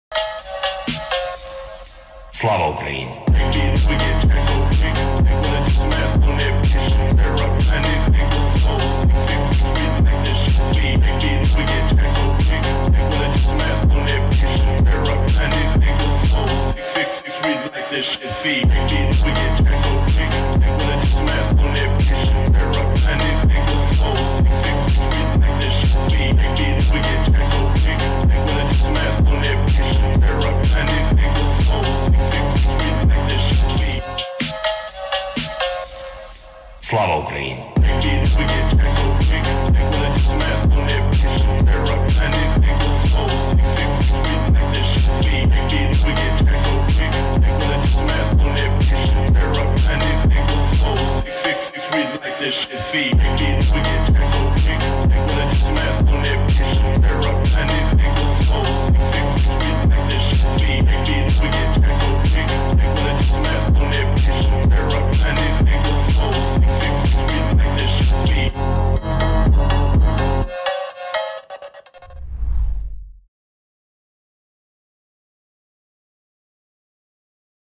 Військові контррозвідники СБУ з початку року «намолотили» десятки одиниць російської техніки
За допомогою ударних дронів наші бійці додали до щоденної статистики втрат окупантів:

▪ 5 танків
▪ 4 великокаліберні гармати «Гіацинт»
▪ 3 БМП та 2 броньовані авто
▪ 4 вантажних автомобілі
▪ 14 плавзасобів
▪ 2 склади з боєкомплектом
▪ вогневі точки мінометного розрахунку

Крім того, у співпраці з бойовими побратимами-артилеристами Сил оборони України було скореговано вогонь і знищено ще 50 одиниць озброєння:

▪14 танків, 10 САУ, 2 самохідні міномети «Тюльпан» калібром 240 мм, 4 гармати, 12 БМП та інших броньованих машин, 14 вантажівок, 2 пункти управління БпЛА «Орлан-10», станцію РЕБ «Р-934БМВ», понтонну переправу, вежу з розвідувальним обладнанням. І на додачу – влаштували феєрверки на 12 складах з боєкомплектом.

На відео – результати роботи підрозділу лише за останні декілька днів.